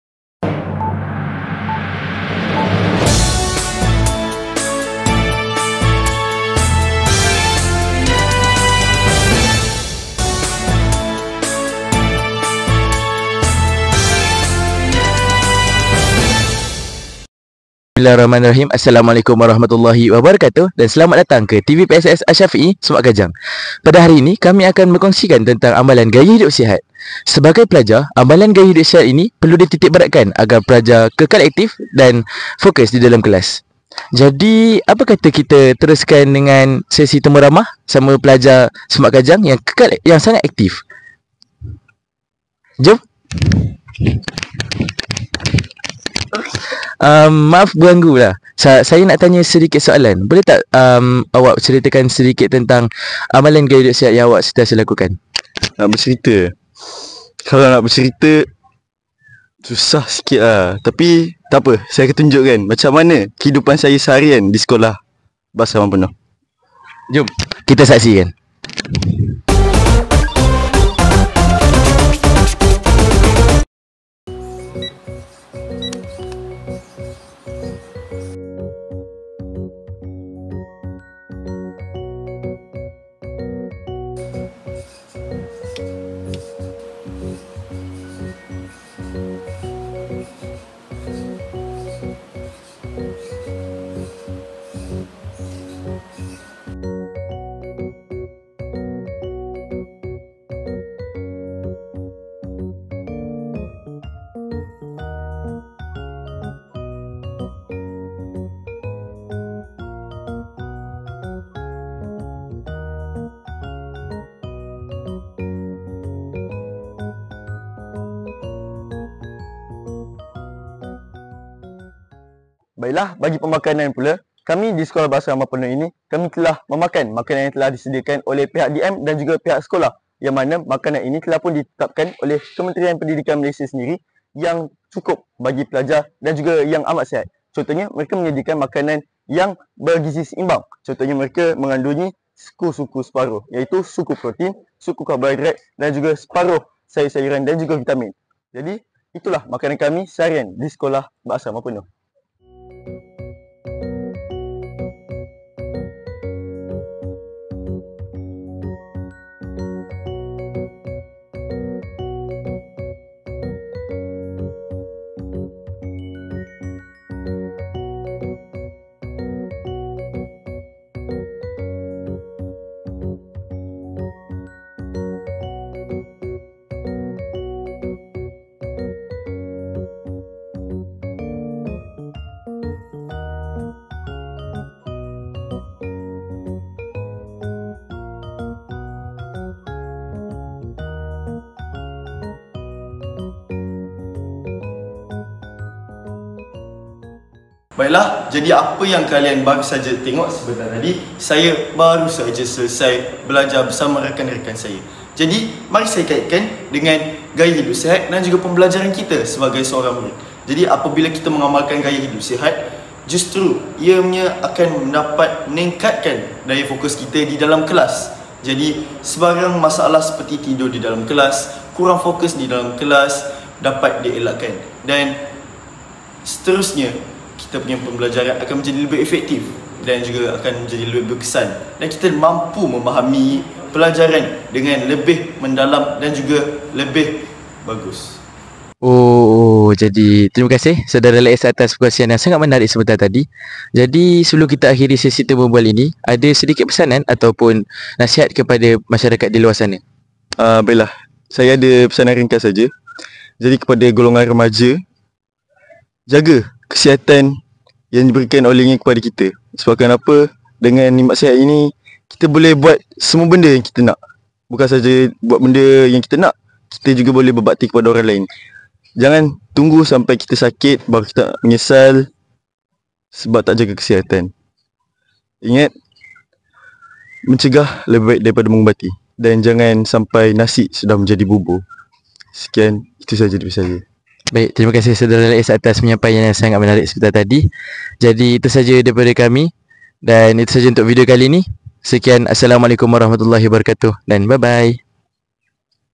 PERTANDINGAN VIDEO TERBAIK TV PSS PERINGKAT DAERAH HULU LANGAT 2024 | GAYA HIDUP SIHAT | TV SYAFIE
"Curious about how to start a healthier and more balanced lifestyle as a student? Join us in this video with Lais, a passionate student committed to embracing healthy habits every day.

From exercising on the school field to balancing study and rest, Lais shows that small steps can lead to big changes. As the saying goes, 'A healthy body, a healthy mind' — let’s work together to build a healthier and brighter generation!